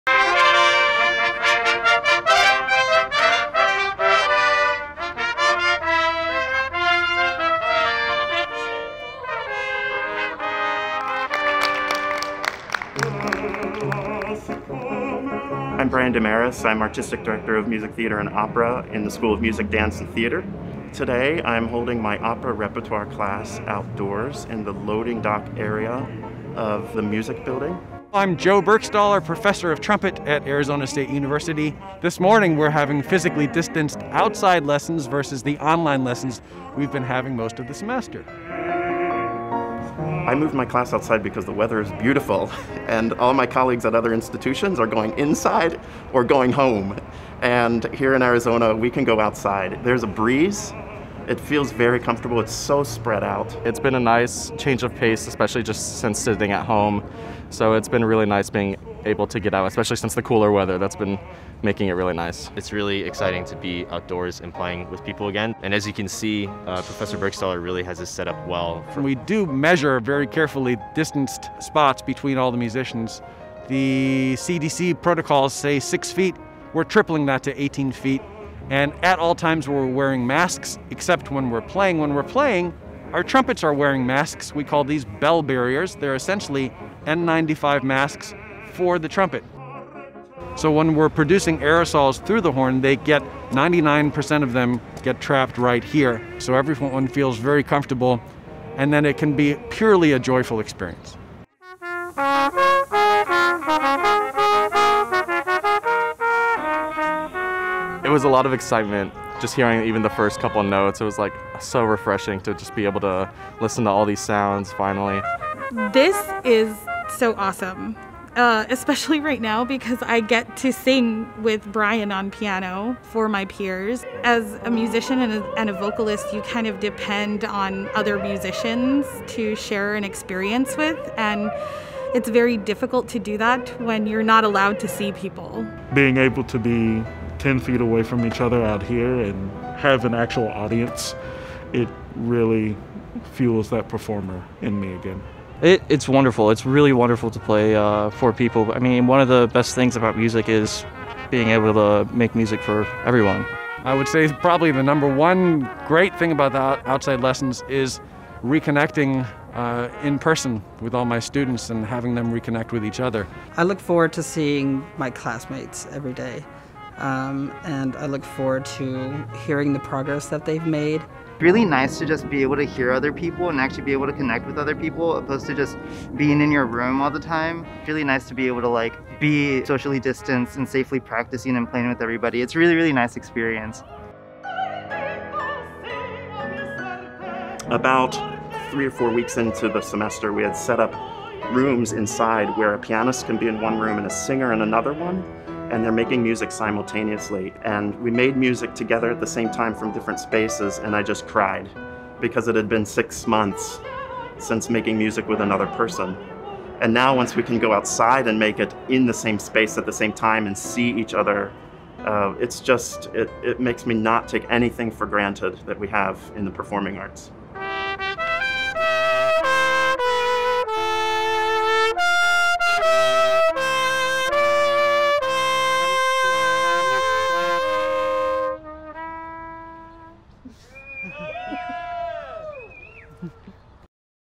Outdoor Classes at Arizona State University (ASU)
Amid the pandemic, faculty in the School of Music, Dance and Theatre in the Herberger Institute for Design and the Arts have found creative ways to unite students. Joe Burgstaller, associate professor of trumpet, and Brian DeMaris, associate professor of music, opera and theater, have taken their classes outdoors. Taking precautions, including spacing of up to 18 feet between individuals and coverings for both faces and trumpets at all times, students can perform at ease and enjoy connecting with their peers. This also enables small audiences to safely gather and opens a channel of creativity during a challenging time.